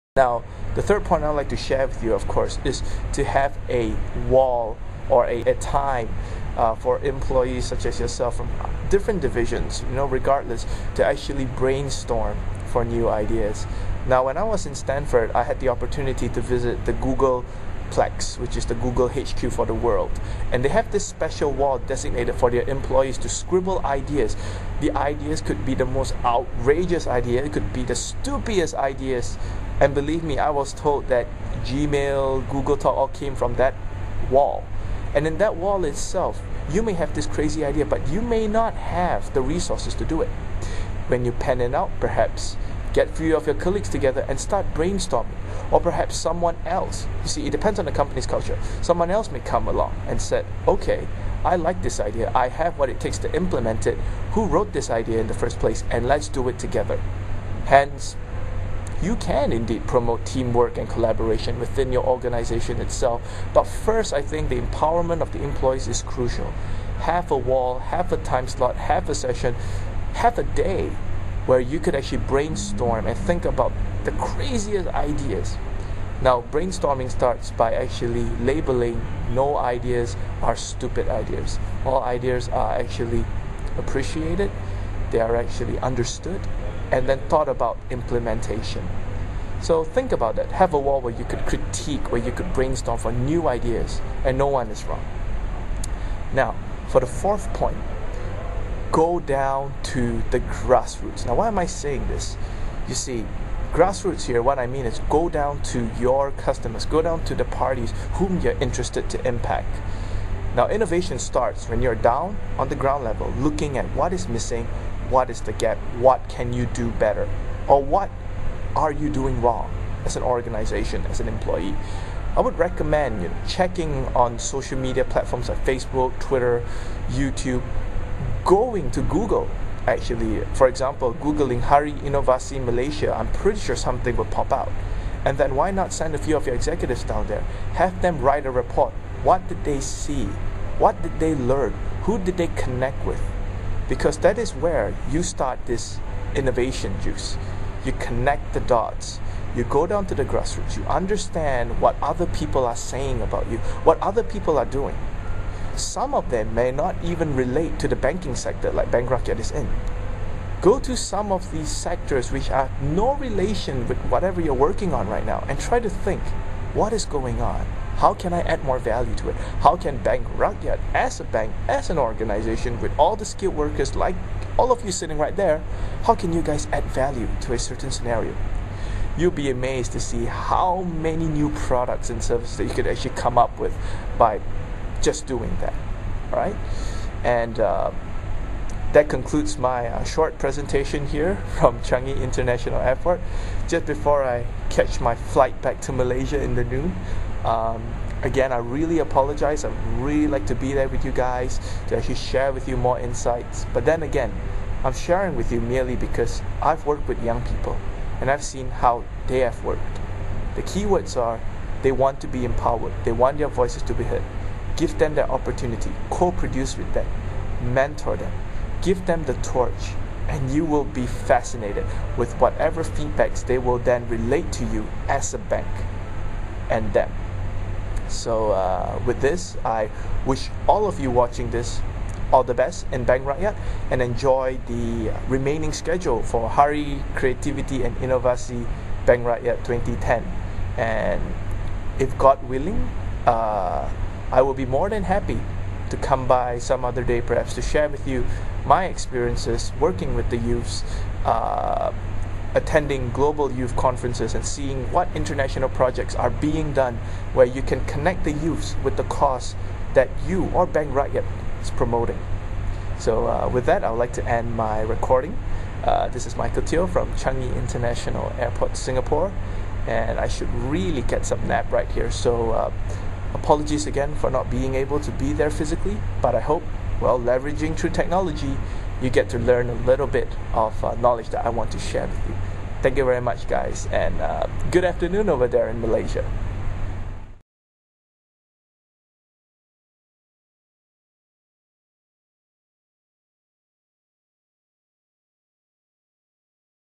Speech for 'Creativity & Innovation Day' @ Bank Rakyat 2010 - Part 2
Michael Teoh speaks to Bank Rakyat in Malaysia from Changi International Airport Singapore about cultivating the Innovation Culture in the Workspace with Youths. Michael was invited to speak on behalf of the World Creativity & Innovation Day, which is celebrated on the 21st of April every year, being led by Bank Rakyat in Malaysia for 2010.